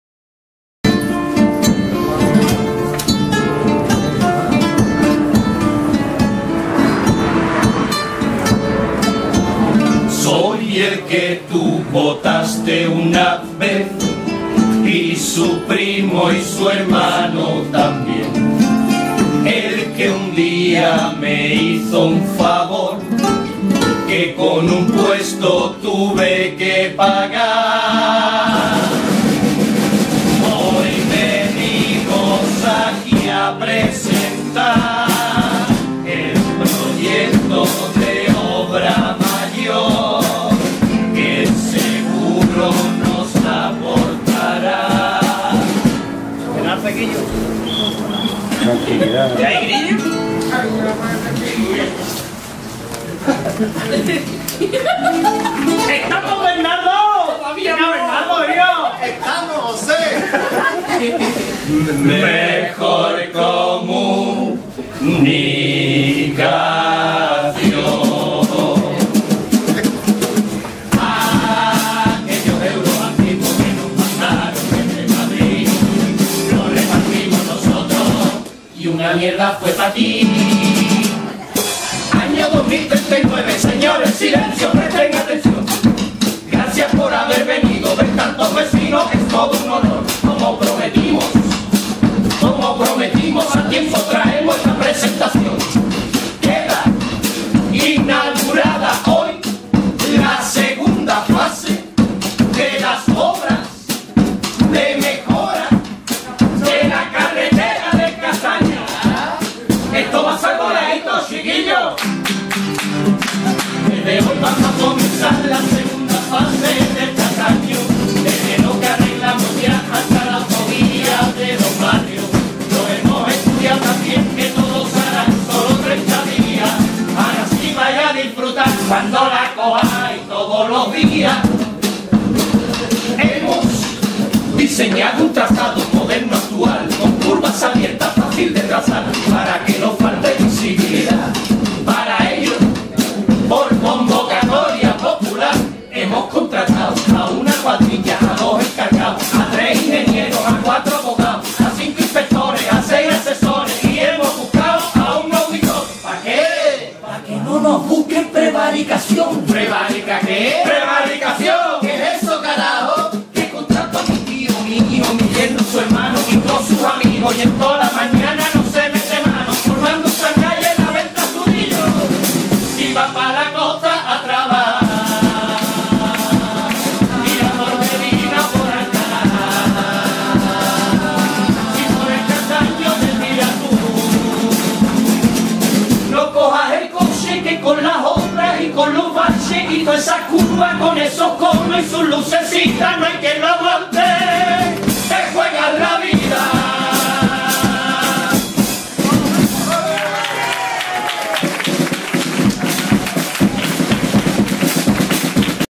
San José de Malcocinado (Cádiz) Carnaval 2019 CHIRIGOTA Esta comparsa pasa por un bache 1
Esta chirigota es de Benalup-Casas Viejas.
Actuación en el Bar La Parada de San José de Malcocinado que es una pedanía de Medina Sidonia